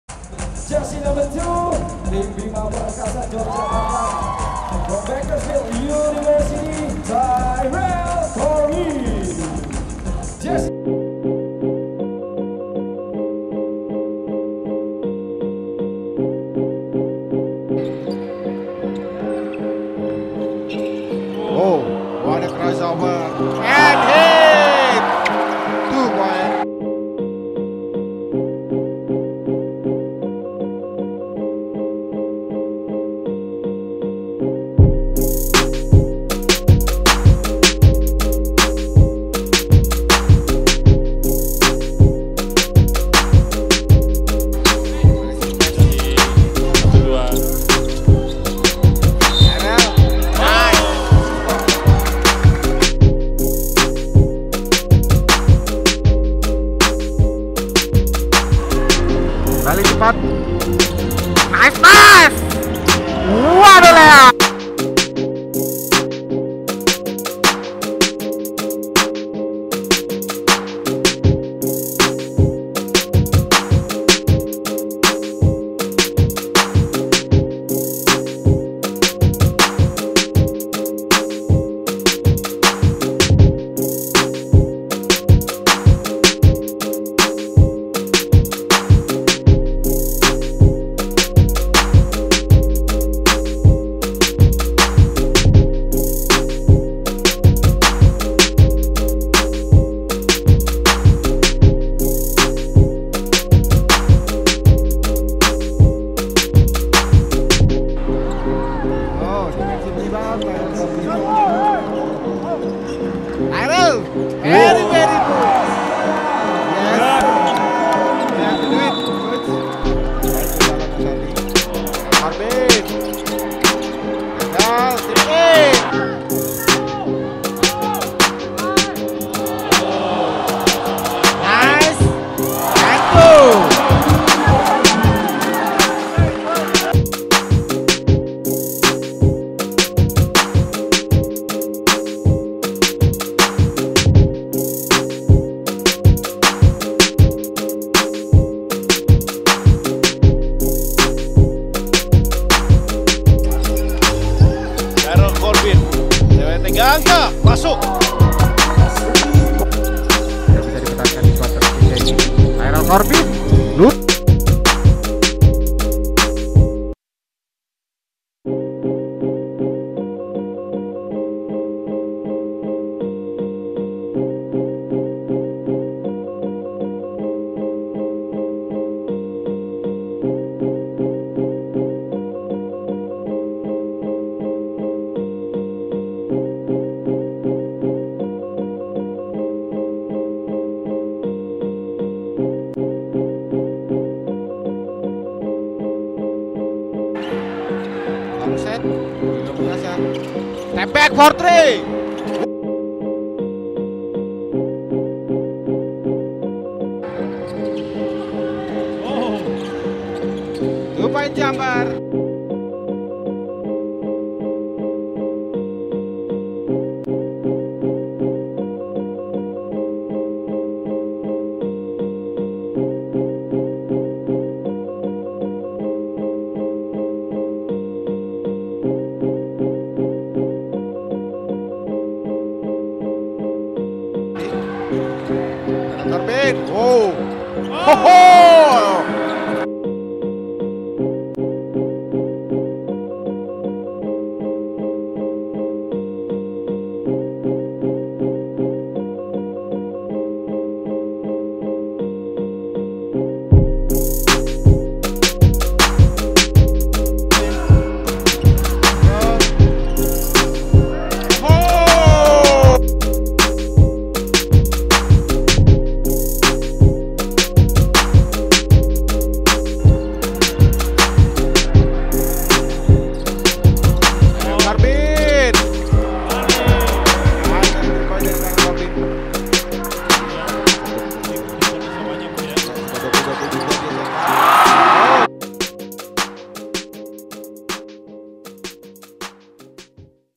Tyrell Corbin 2017 IBL Highlights
Highlights from a few games in the 2017 IBL Season.
6 ft (185 cm) 185 lb (84 kg)

Top 5 in the League in Scoring (24.86)
Led IBL in Steals per game (2.93)
Named 2017 IBL Pertalite All Star